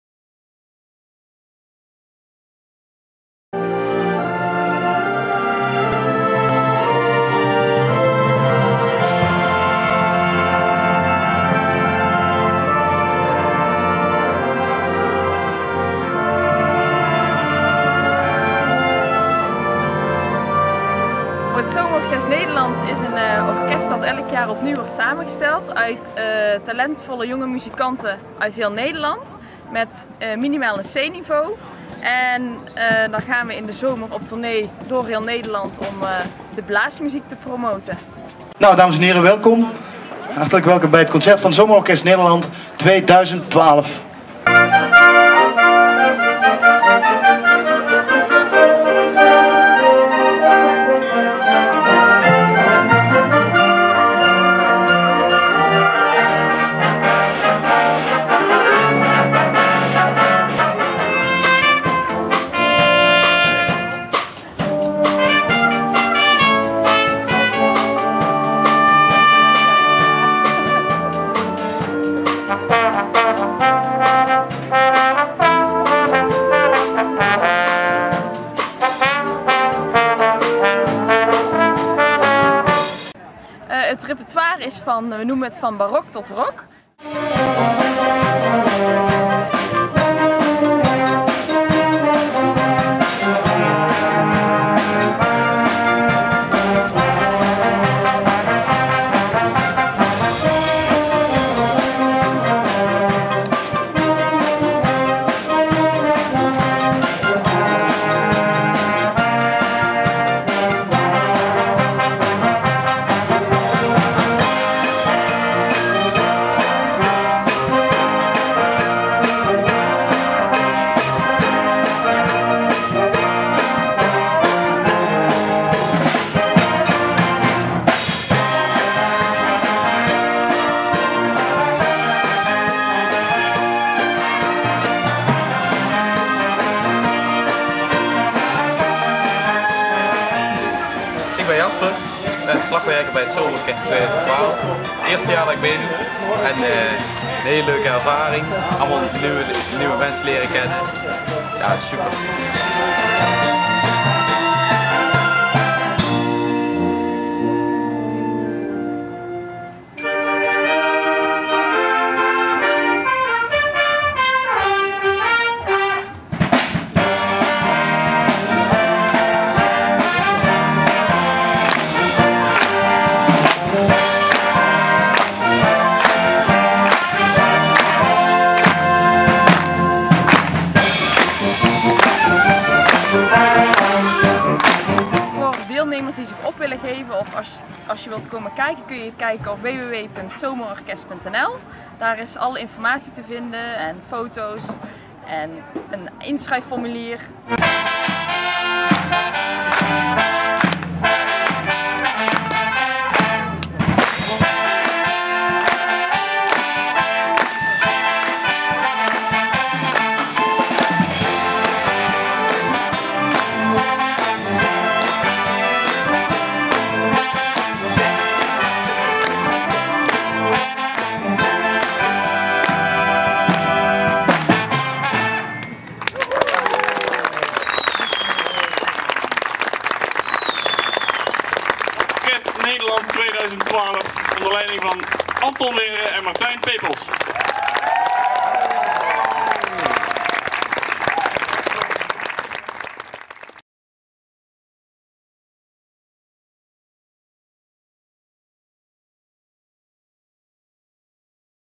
ZomerOrkest NL 2012
Zomer Orkest Nederland trad op op 28 juli 2012 op de Floriade in Venlo.